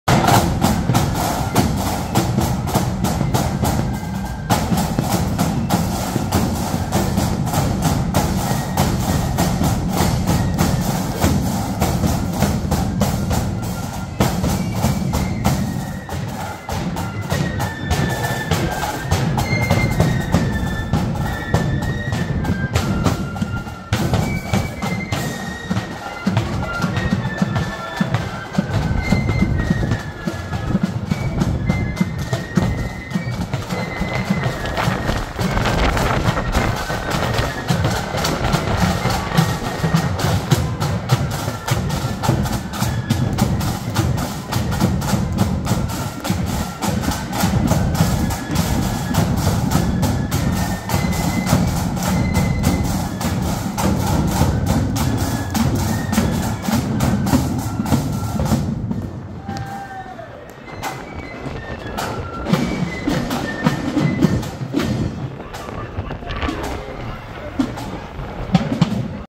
Prince of Orange parade - David Law memorial band parade(part of parade) 23rdmarch 2024
Prince of Orange Flute band - David Law memorial band parade(part of parade) Partick protestant boys fb / Cambuslang Britannia fb  23rdmarch 2024